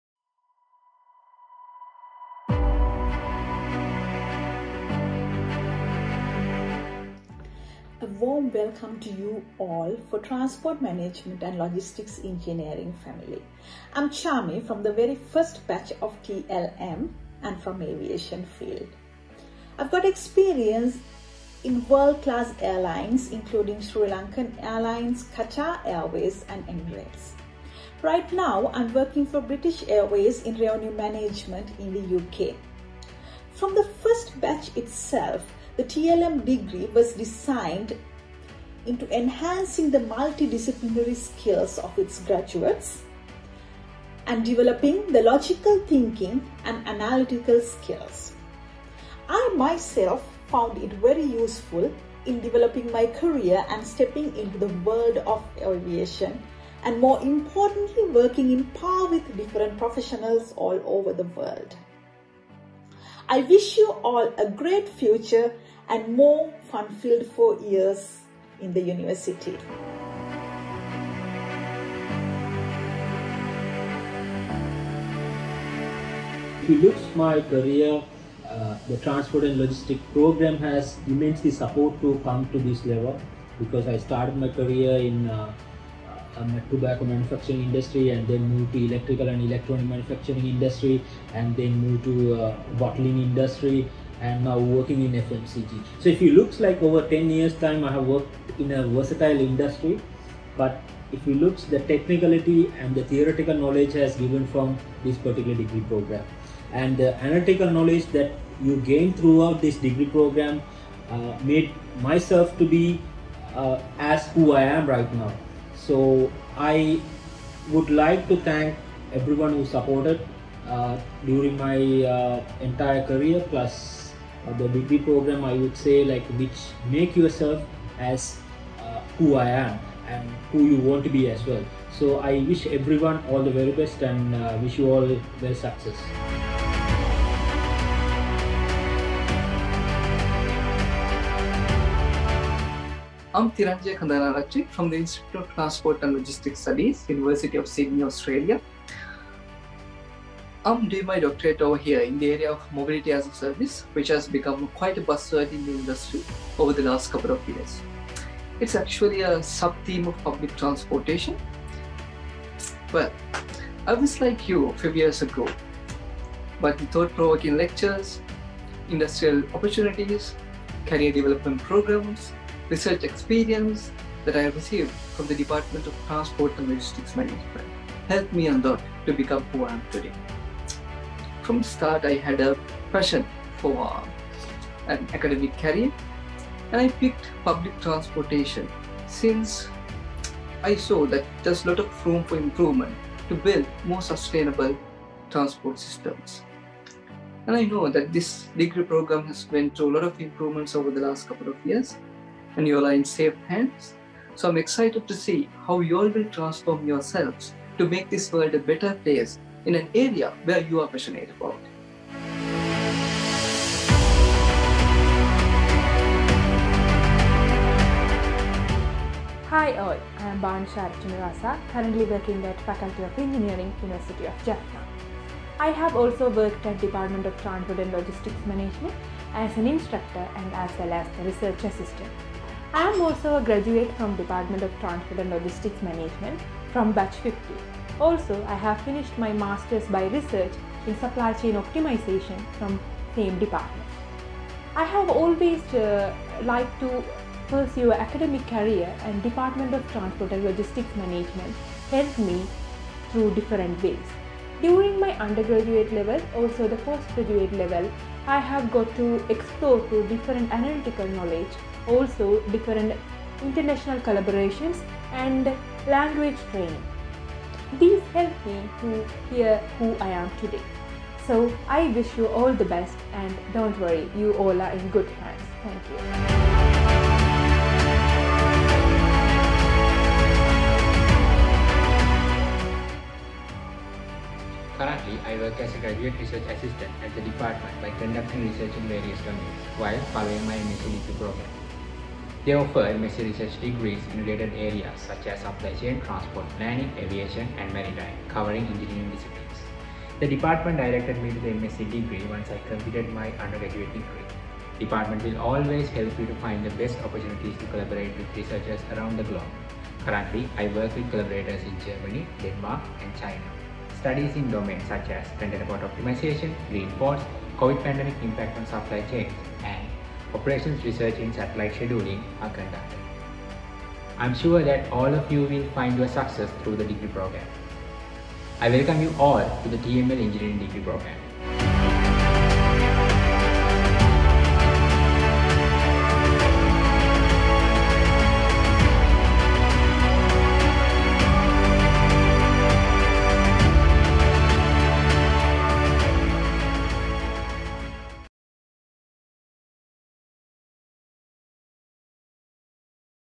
Transport Management & Logistics Engineering - A Bridge to Win the World
"Nothing can be achieved in life without taking that first step"

Time has arisen to take the first step.

The Department of Transport & Logistics Management has endowed skillful, enthusiastic and successful graduates to multidisciplinary fields across the world, over the years.

Now the department is ready to cater the new industry demands by converting its degree program to Transport Management & Logistics Engineering.
Transport Management & Logistics Engineering graduates are the next generation ready to conquer the world of transport and logistics.